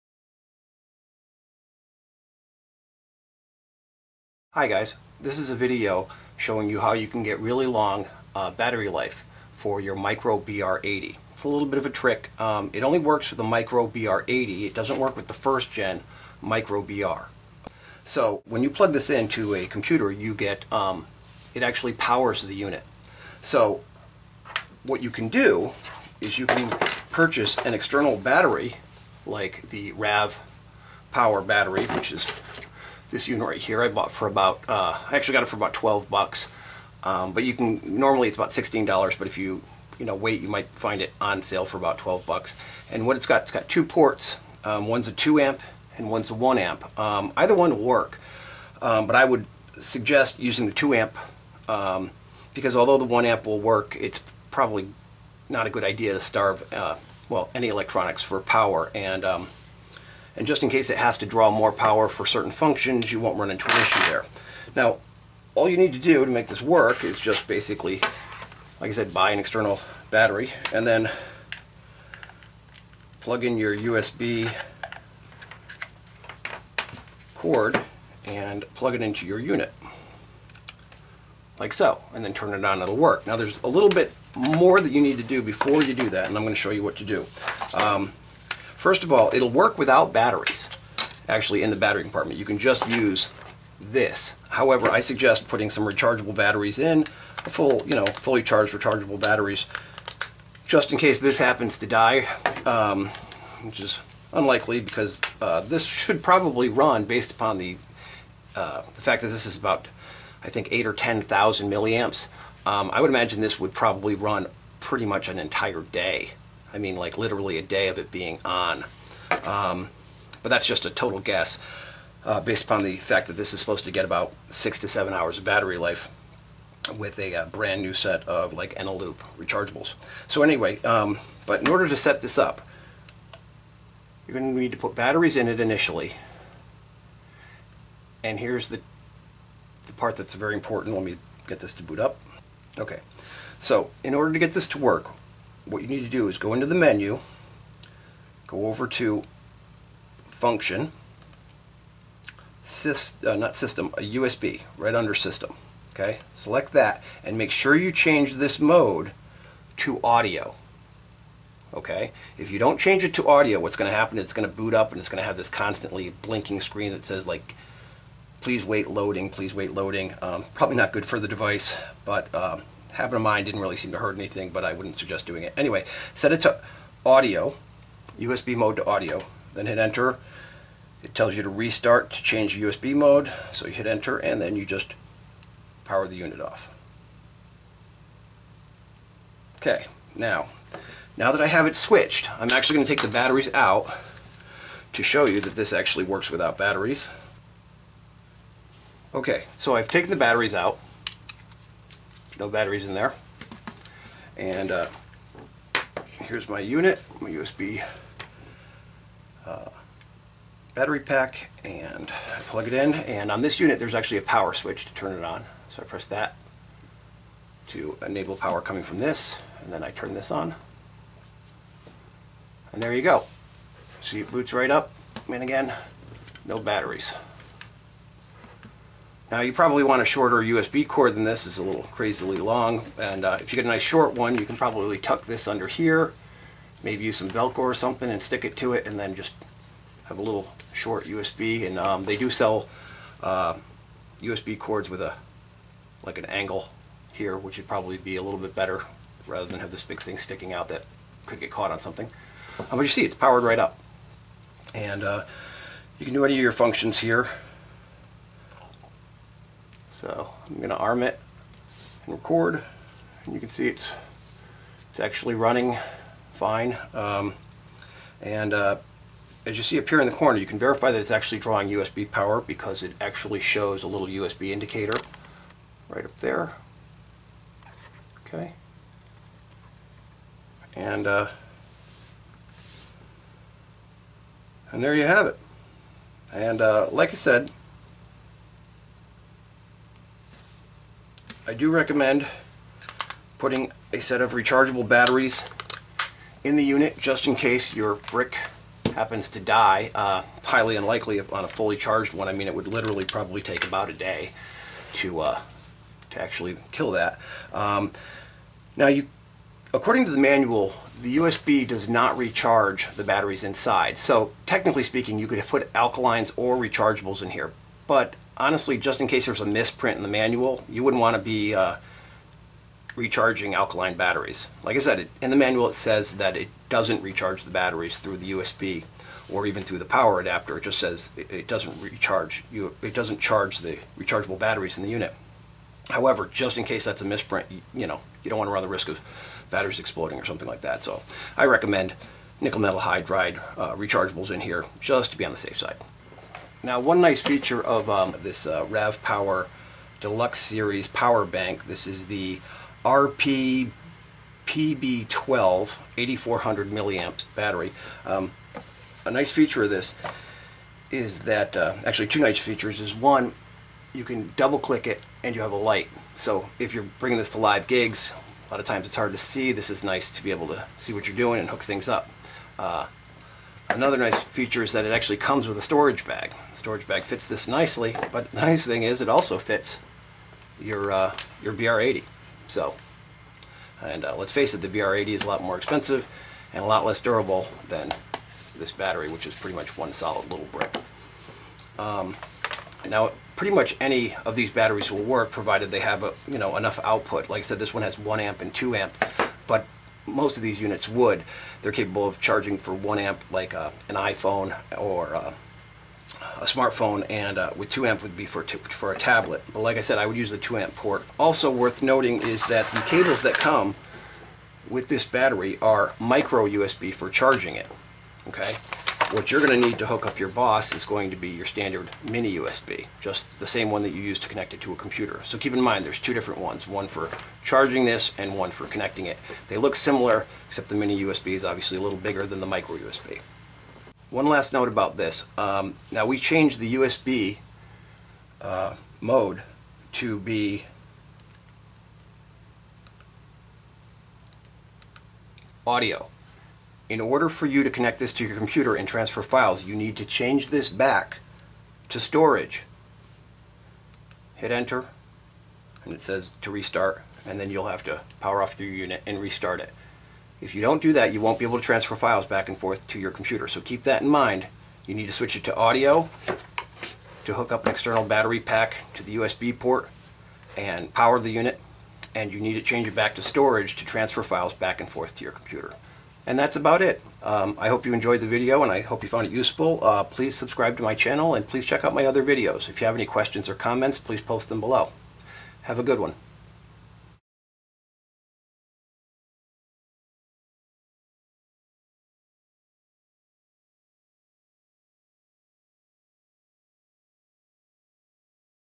Boss Micro BR BR-80 Trick - How to use external battery for 24 Hour Battery Life !
This video shows how to hook up an external USB power brick to your BOSS BR BR-80 to achieve upwards of 24 hours of battery life (more or less depending on the maH rating of the power brick you connect to it.